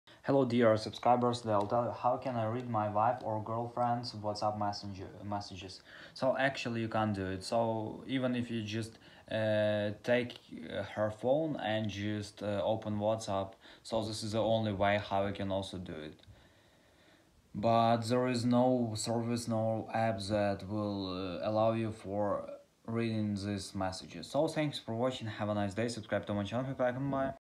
How Can I Read My Wife Or GirlFriend’s WhatsApp Messages
If you don’t know How To Read My Wife Or Girlfriend’s Messages In WhatsApp, this video is for you.